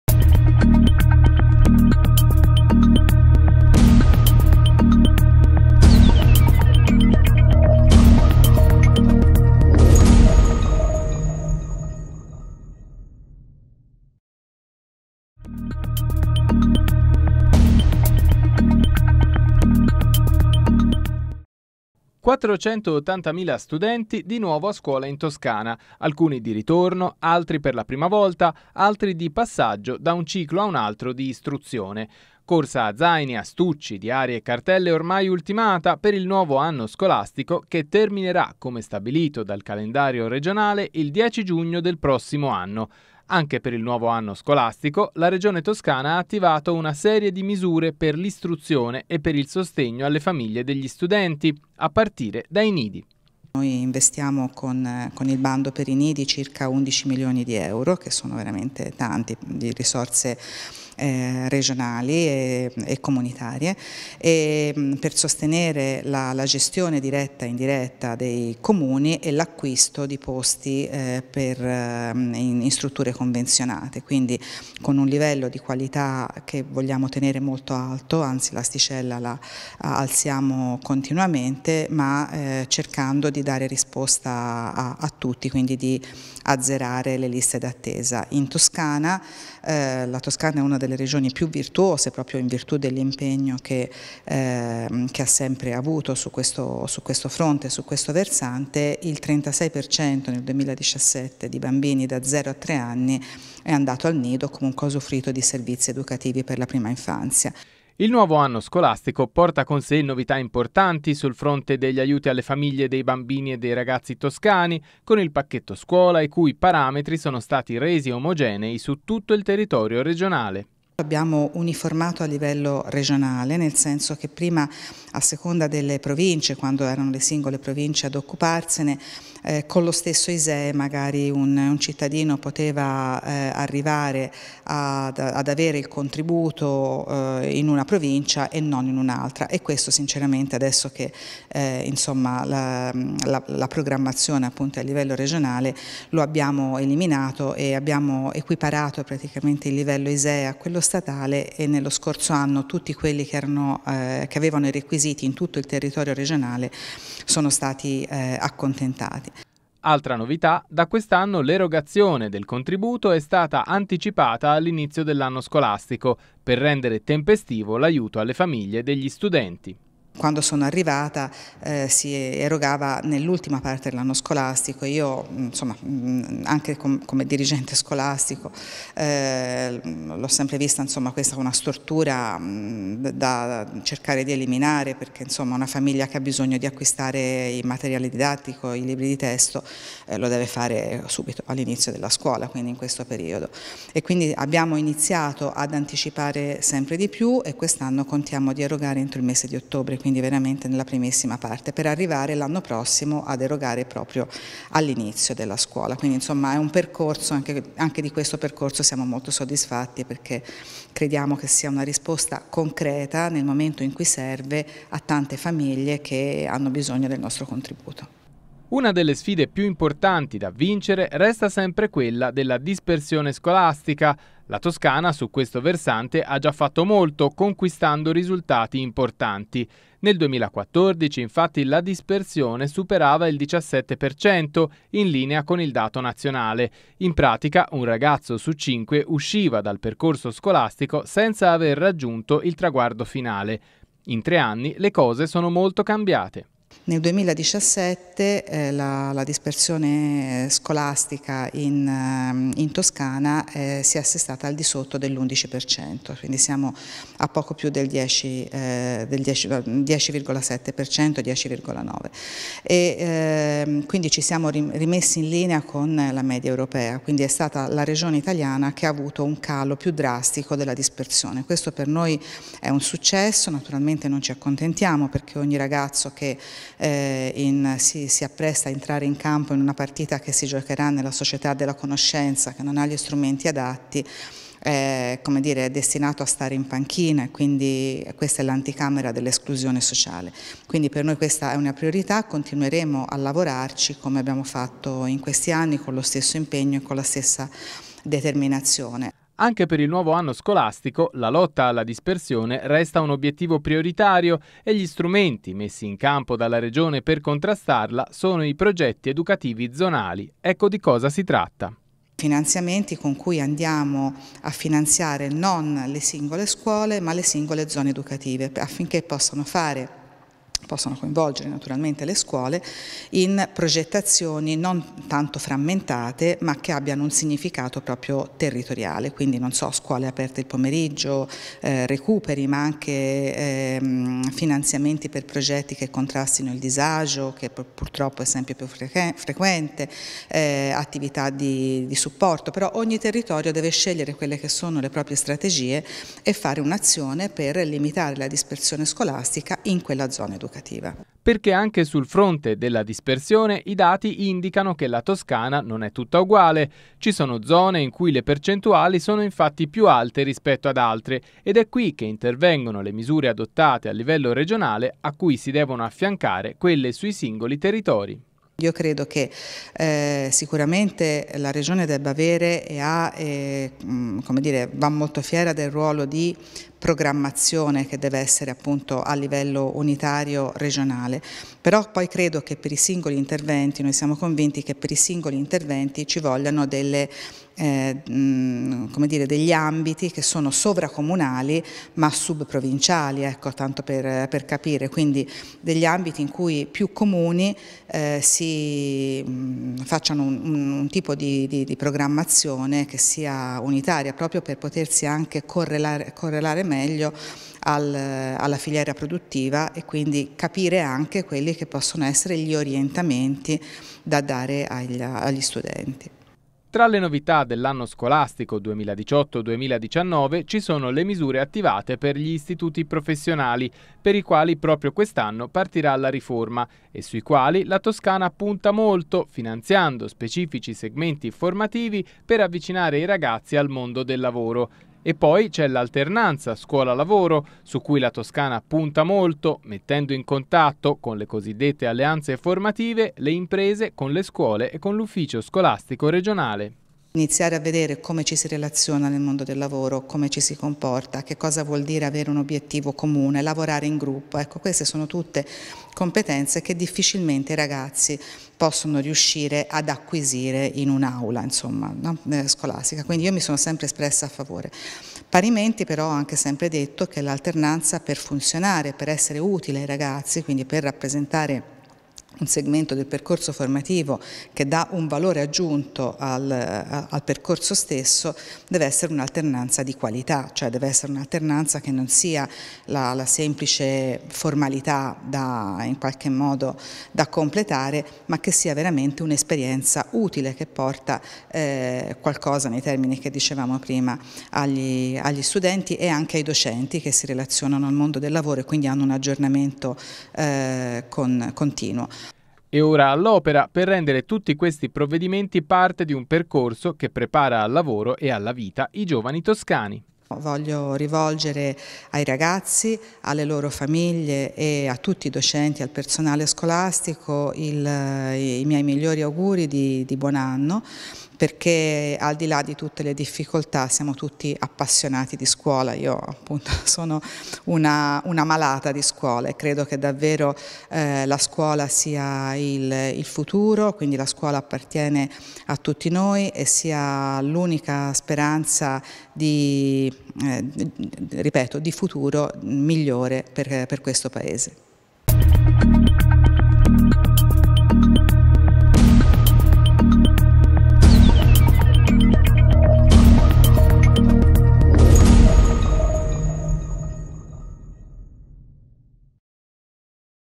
Gli studenti toscani tornano in classe
Le novità e le misure attivate dalla Regione a sostegno di studenti e famiglie per il nuovo anno scolastico.